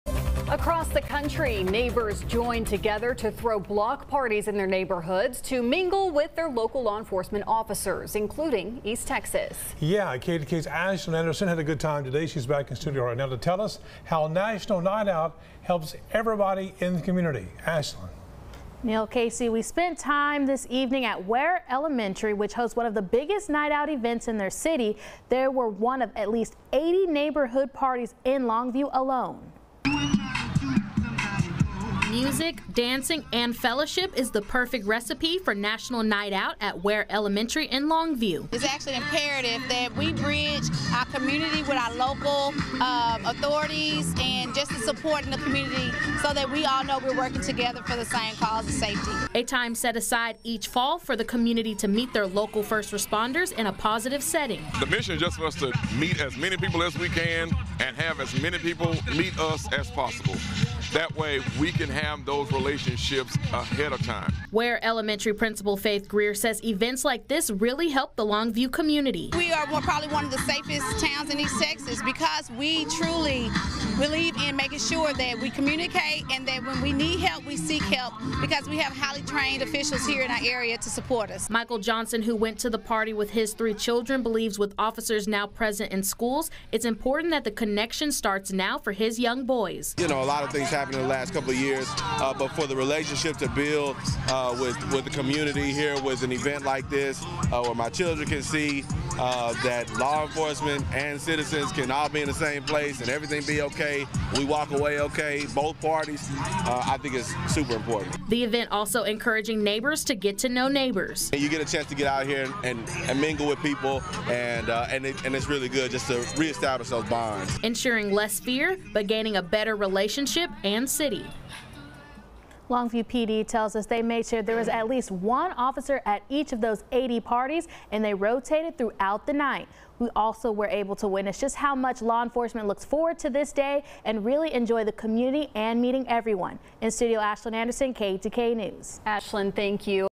Ware Elementary hosts huge National Night Out Event
Music, dancing and fellowship is the perfect recipe for National Night Out at Ware Elementary in Longview.